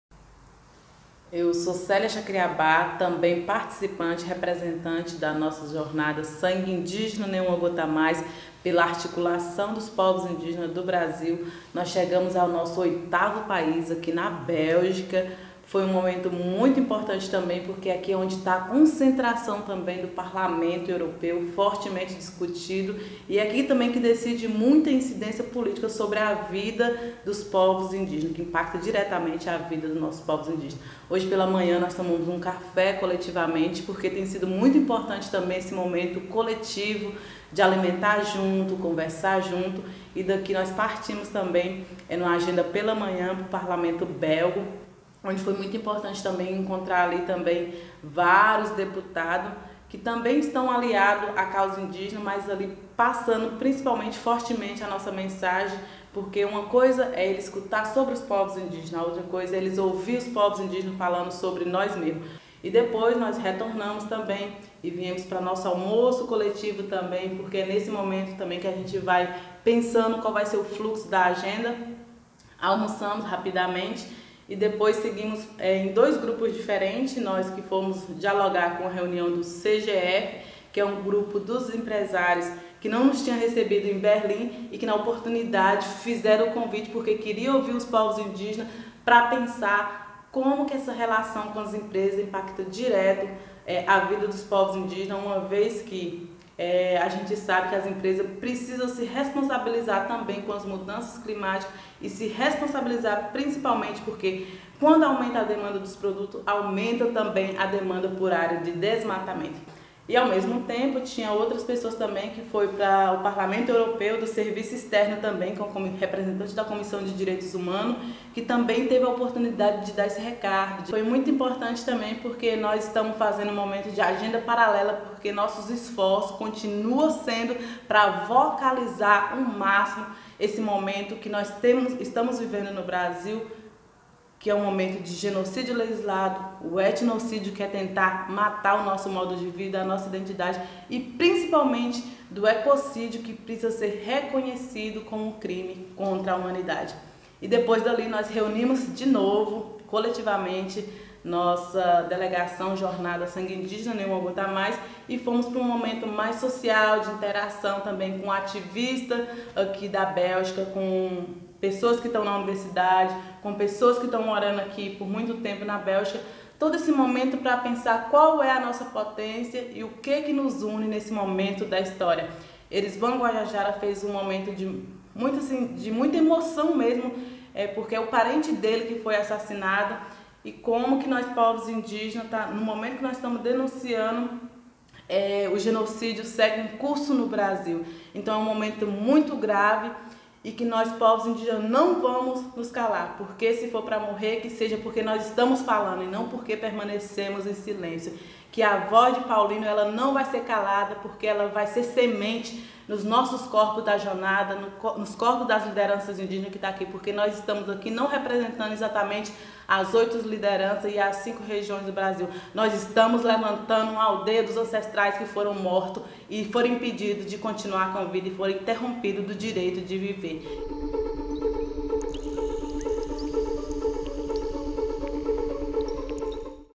Diário de Bordo #18 Sangue Indígena: Nenhuma Gota a Mais
Célia Xakriabá conta como foi o primeiro dia de agendas externas na Bélgica, que já teve reunião com setores do Parlamento Europeu, com empresas multinacionais e o encontro com apoiadores locais.